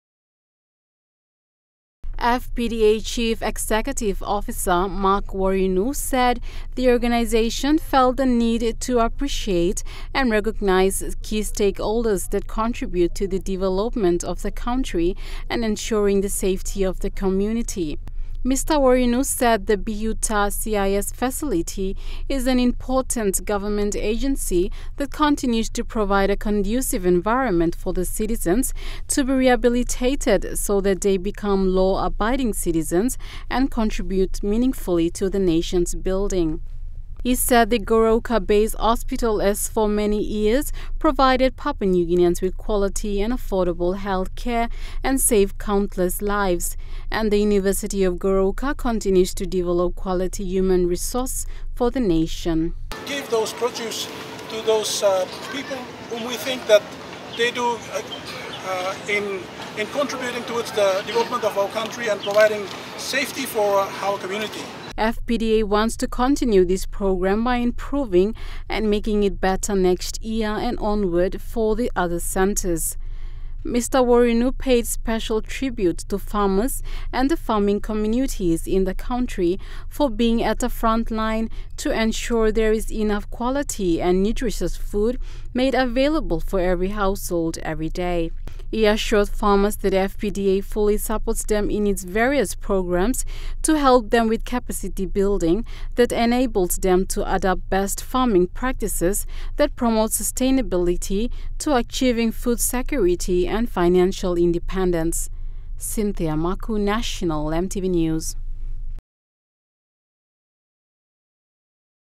FPDA COMMITTED
The Fresh Produce Development Agency (FPDA) had invited three key government agencies, the Bihute Correctional Service Institute, Goroka Base Hospital, and the University of Goroka to observe the World Food Day celebrations on the 16th of October which was celebrated in Goroka Eastern Highlands.
FPDA also invited members of the Nagamiufa community living outside Goroka town, which have been displaced by the recent internal conflict, to have a share in the food items that were displayed at the event.